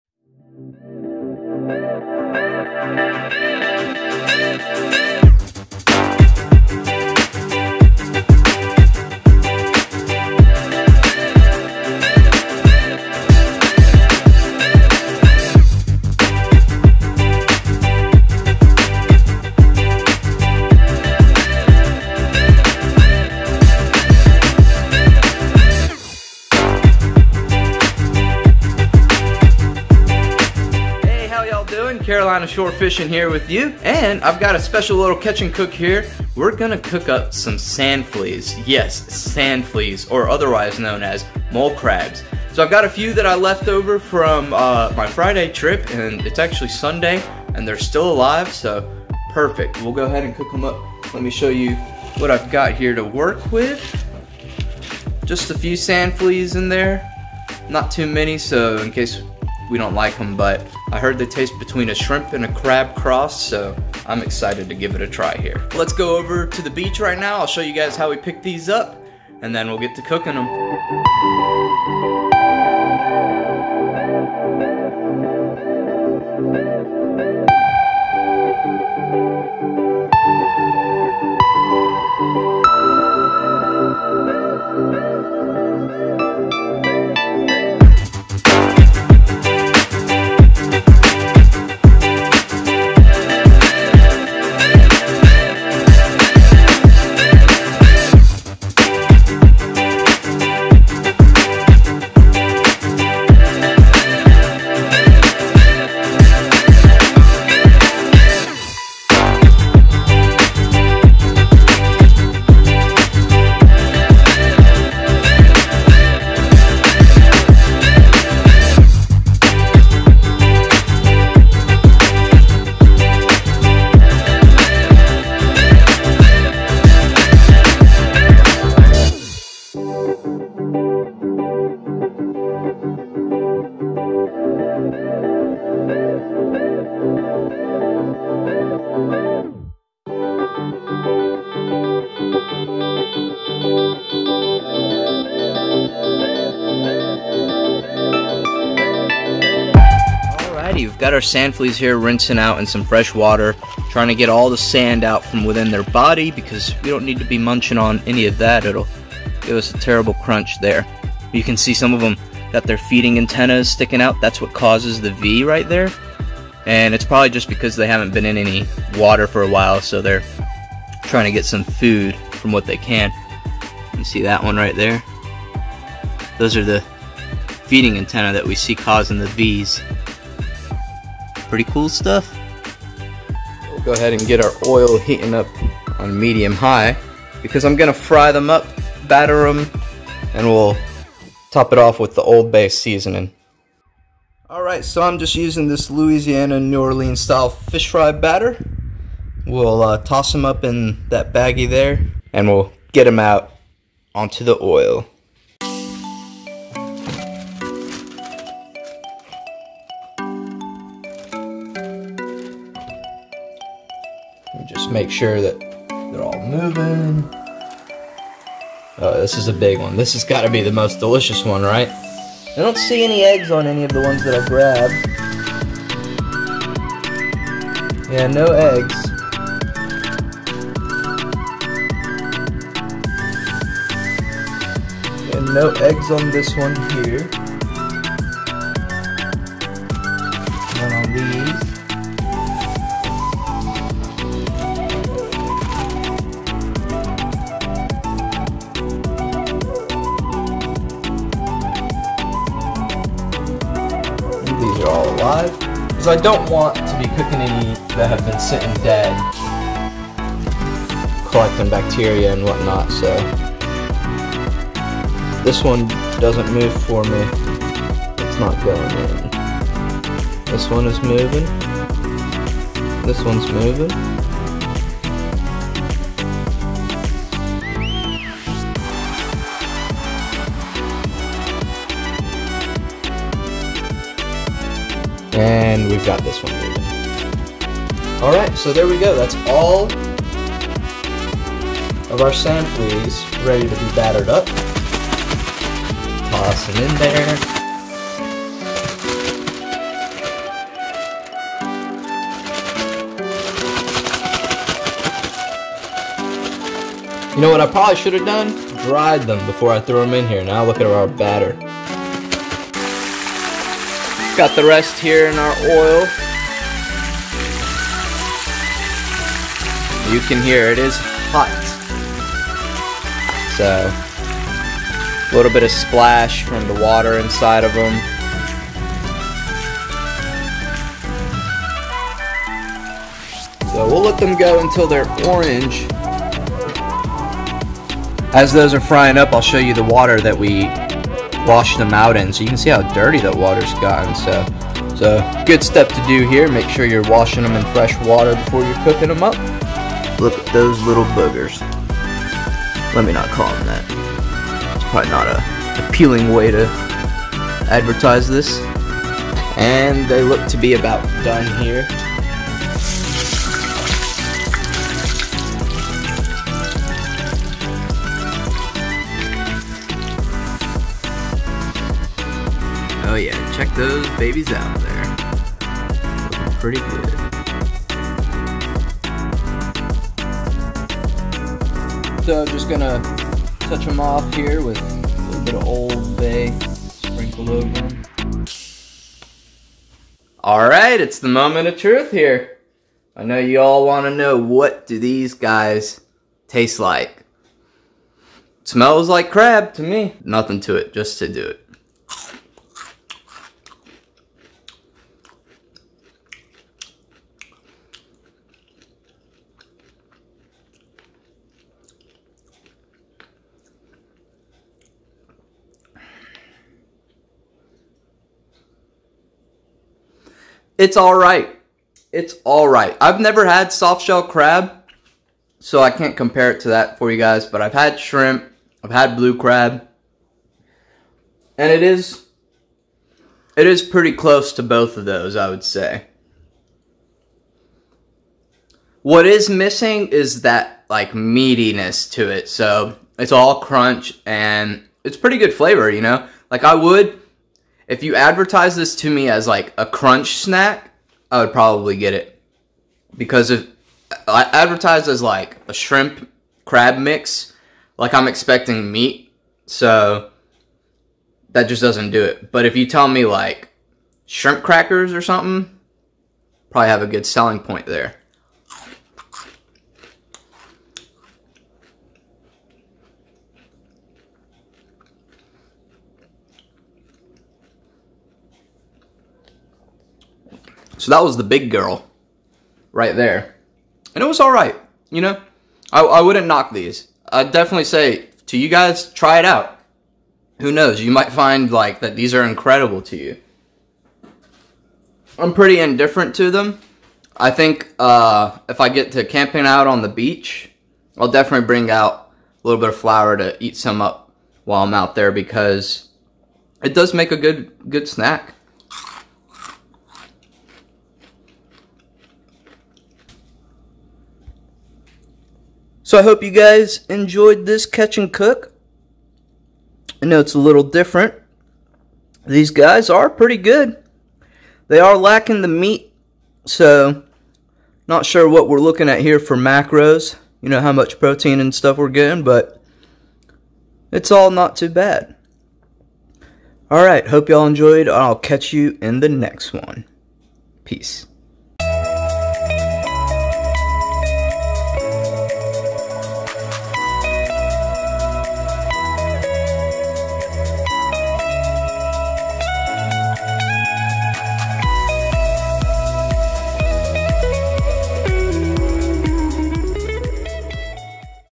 Fried Sand Flea Bites: Catch and Cook
Actually not bad at all! Makes a delicious seafood snack! I would consider this for an appetizer before a seafood dinner!